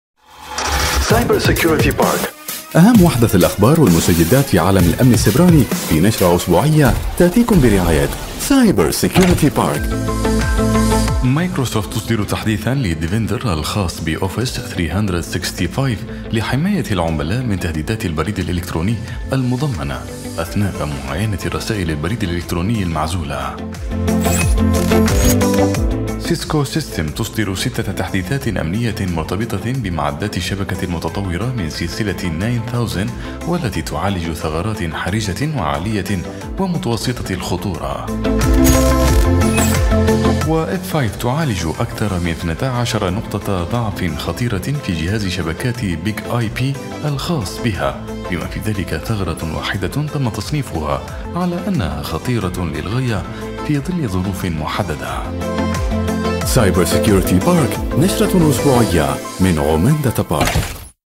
نشرة أخبار الأمن السيبراني الأسبوعية
اخر أخبار الامن السيبراني:

 Microsoft تصدر تحديث لـ Defender  الخاص بـ Office 365 لحماية العملاء

أصدرت Cisco Systems ستة تحديثات أمنية مرتبطة بمعدات الشبكات المتطورة من سلسلة 9000

عالجت F5 أكثر من 12 نقطة ضعف خطيرة في جهاز شبكات BIG-IP الخاص بها